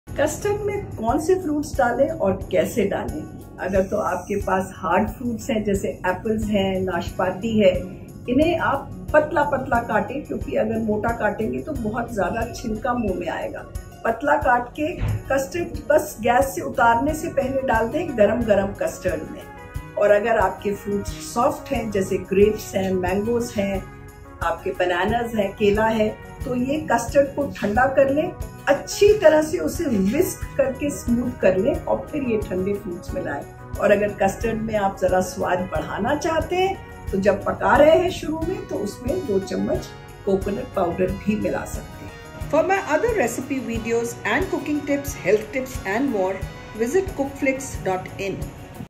How and When to Add Fruits to Custard | Chef Nita Mehta
Add hard fruits to hot custard and soft fruits after the custard turns cold. Adding a little coconut powder while cooking custard gives it a different flavour.